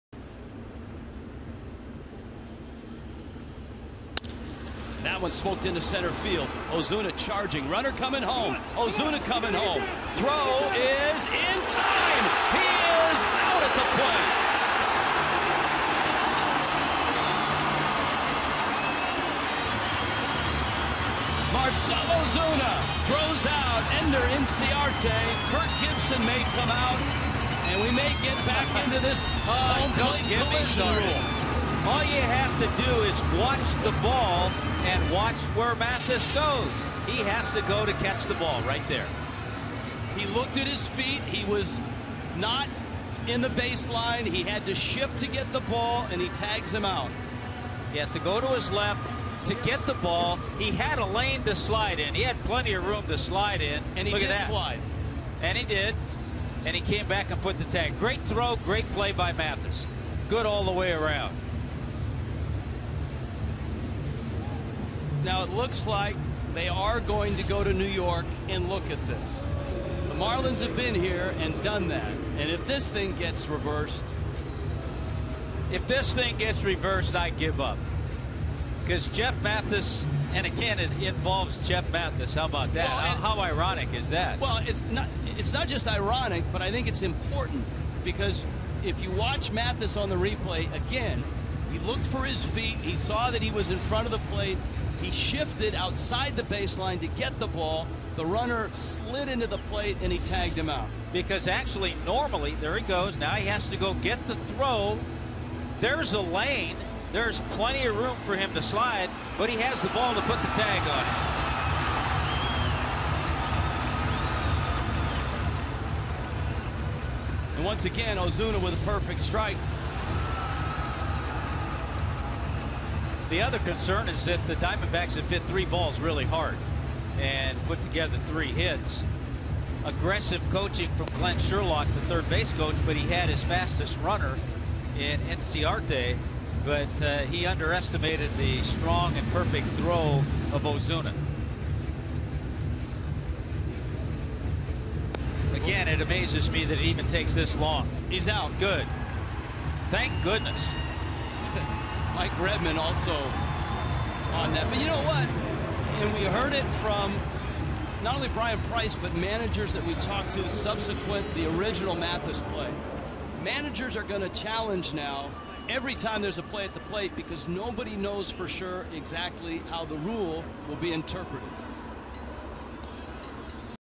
ARI@MIA: Umpires review play at home in the 1st
8/16/14: The umpires review an out call at home in the 1st to see if the Marlins illegally blocked the plate, but the call is confirmed